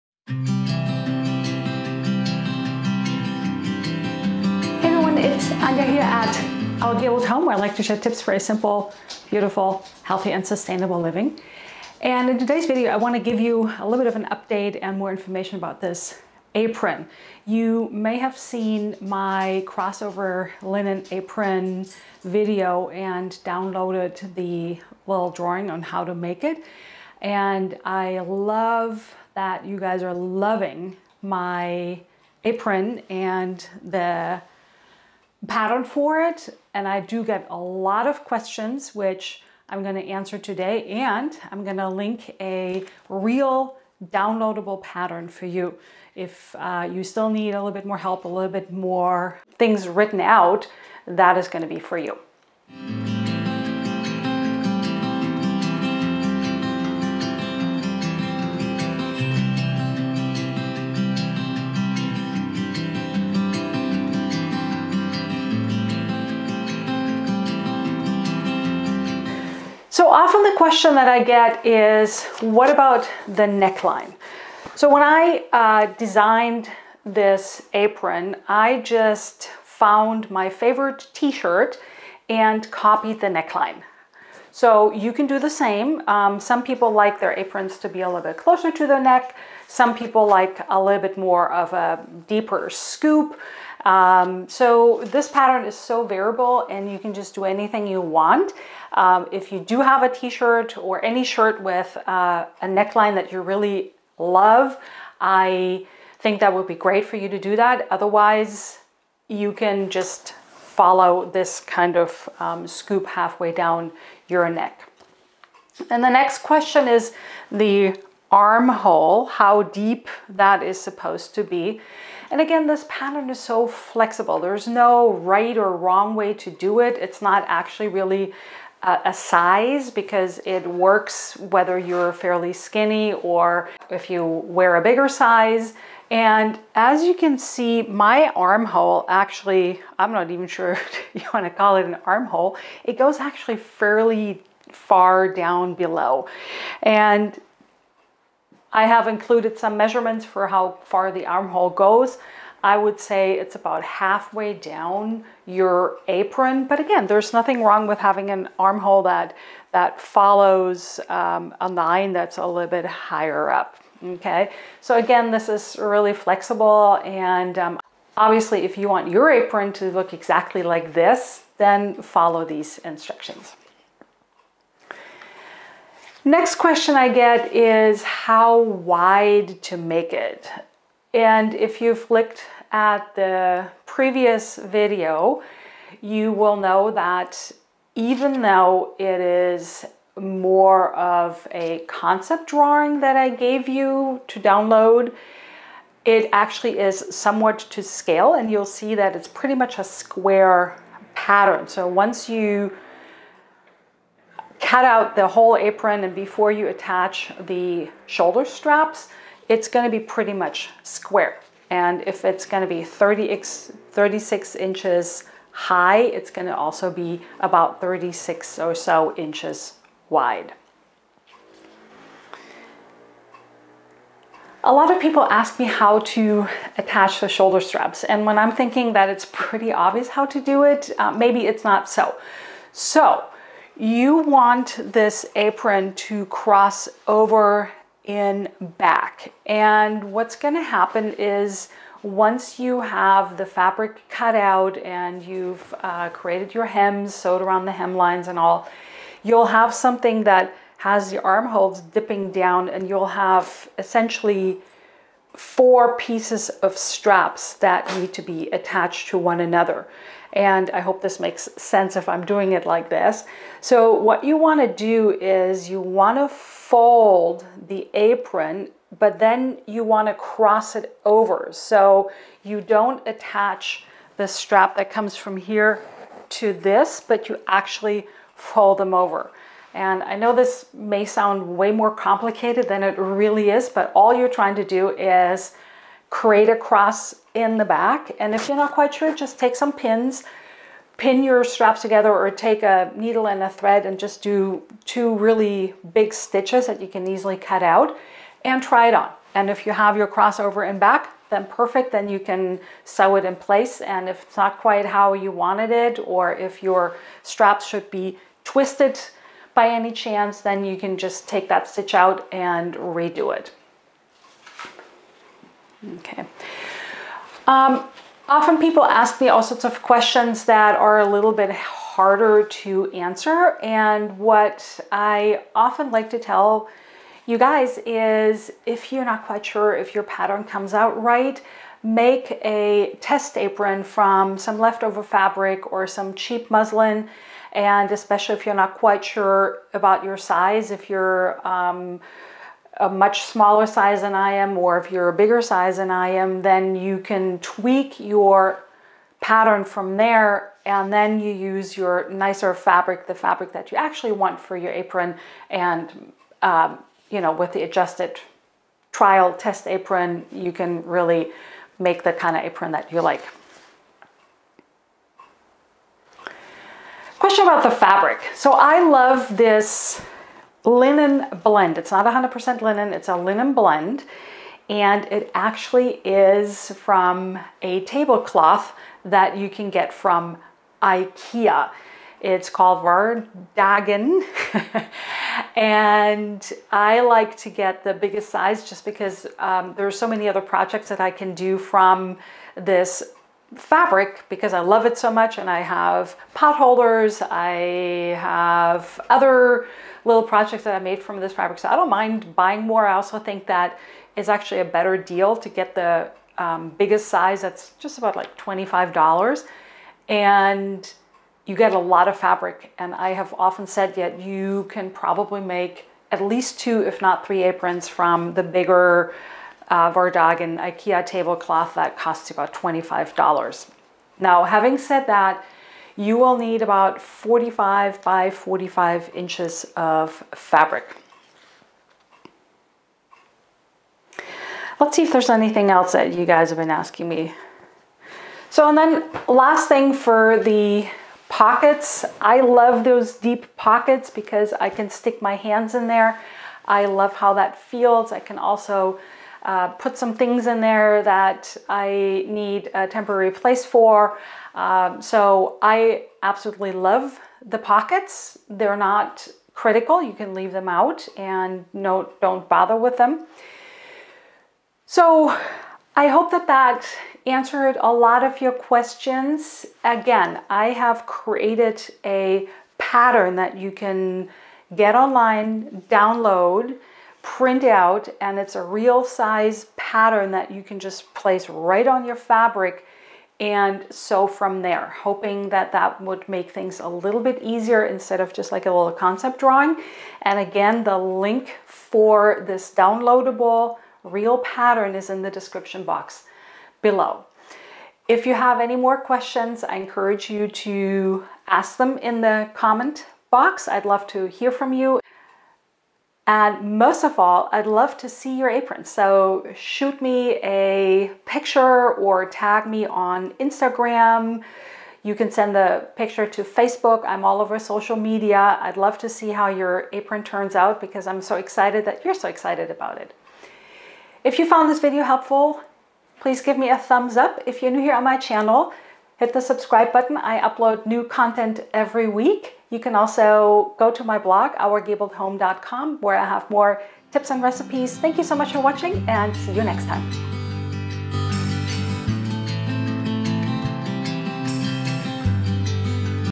Cross-over Apron FAQs & Downloadable Pattern
With my popular cross-over apron, there have been so many questions. In this video, I am answering those asked the most, such as how to make the neckline, arm scoop, the width of the pattern, the fabric etc.